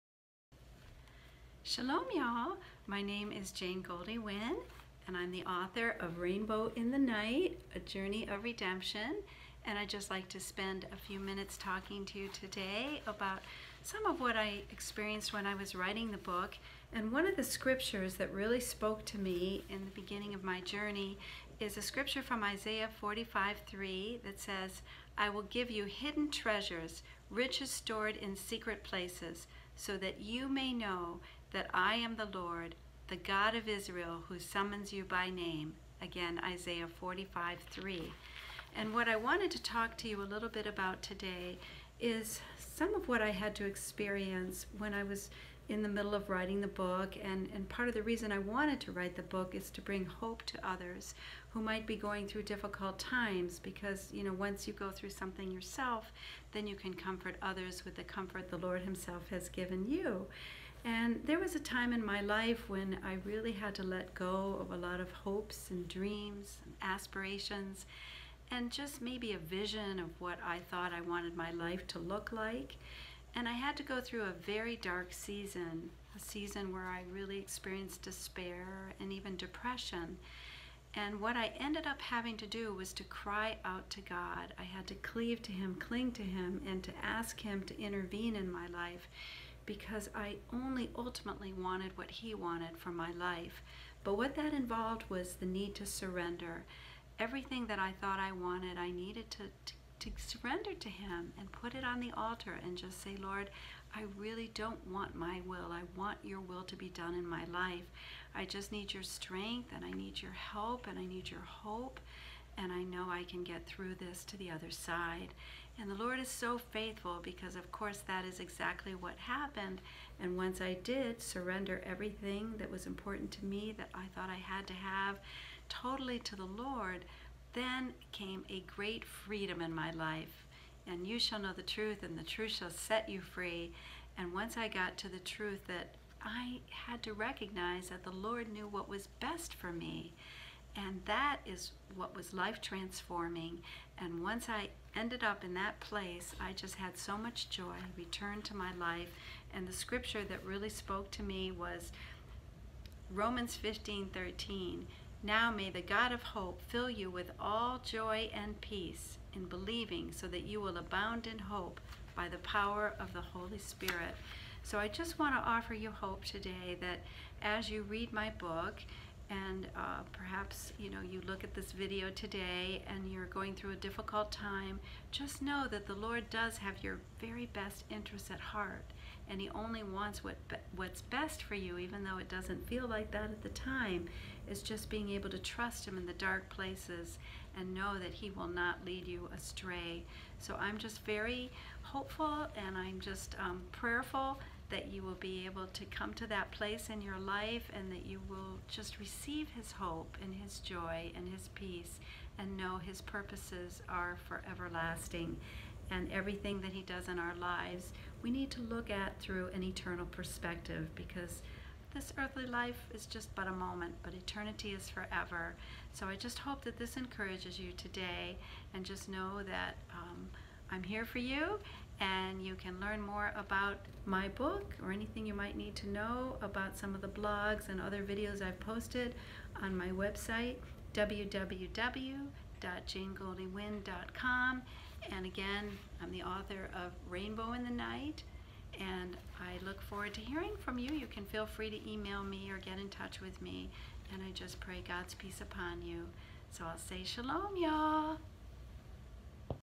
Trust the Lord with His Plan for Our Lives
Jane “Goldie” Winn author of “Rainbow in the Night: A Journey of Redemption” talks about her life and the challenge to surrender hopes and dreams to the Lord. Even though grieving is involved, transformation and joy are gifts we receive when we place our total trust in the Lord. janegoldiewinn.com